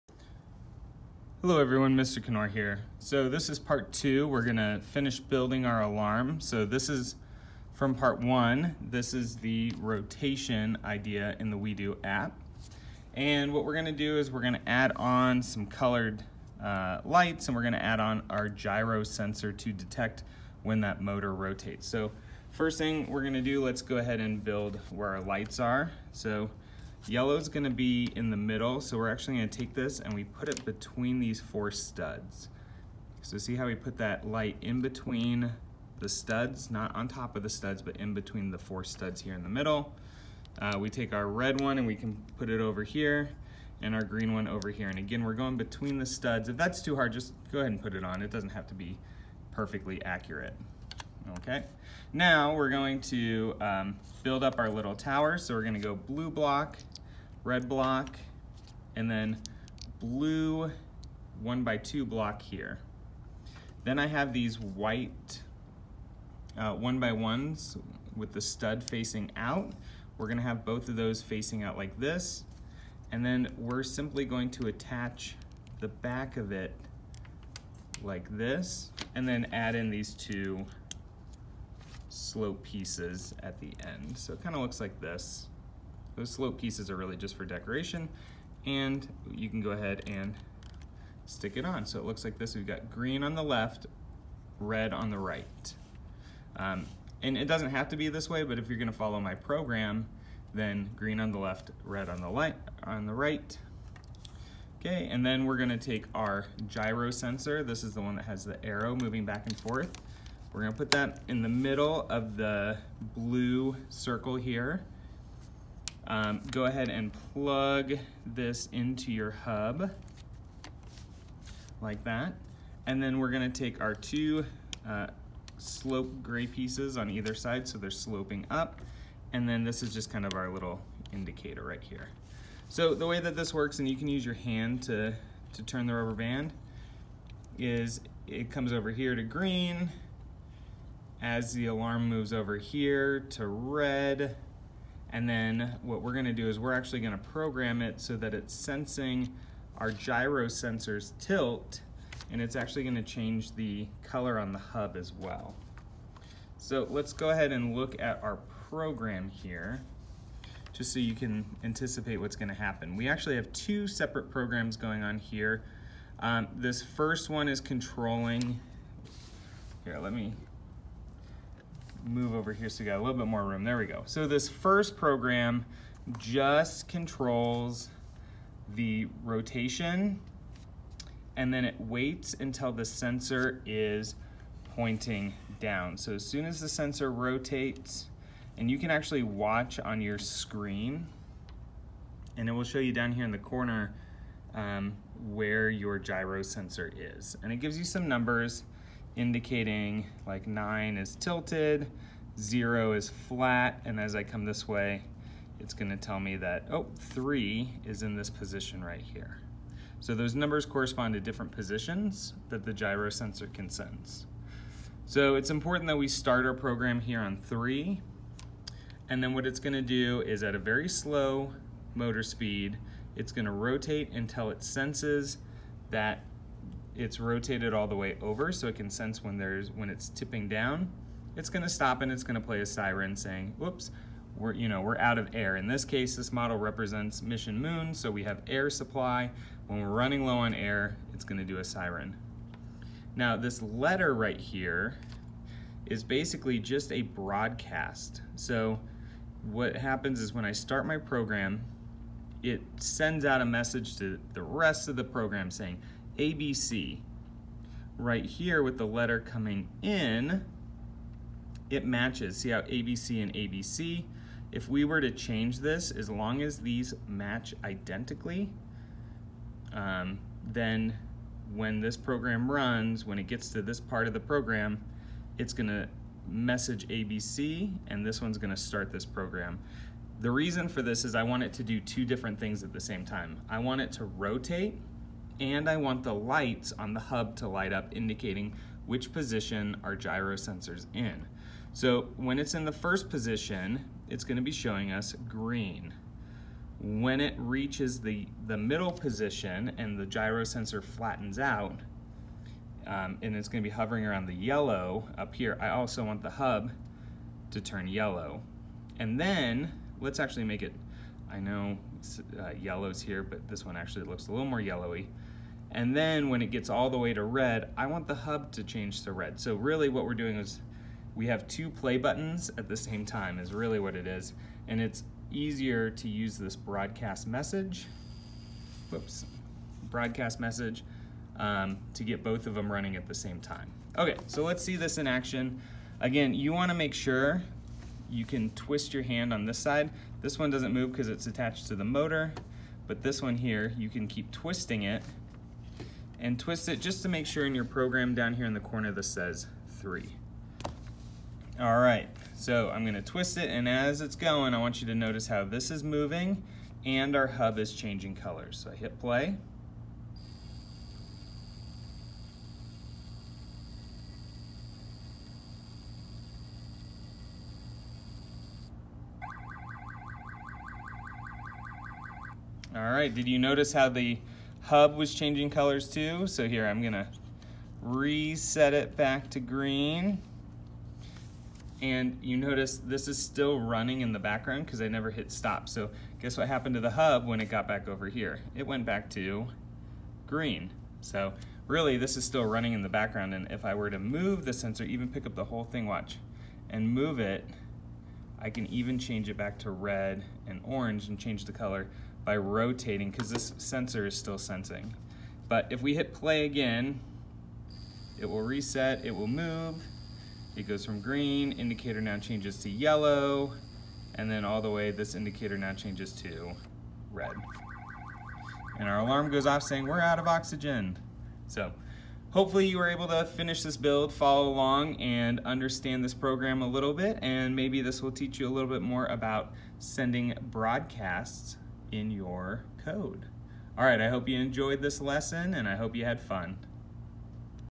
WeDo Alarm 11a building tutorial
This is part 2 of the building tutorial for the WeDo alarm 11a model. This begins with the revolve model already built. There are instructions for the "revolve" in the ideas section of the WeDo app.

Copyright @ [Tim Knorr, 2021]. Any illegal reproduction of this content in any form will result in immediate action against the person concerned.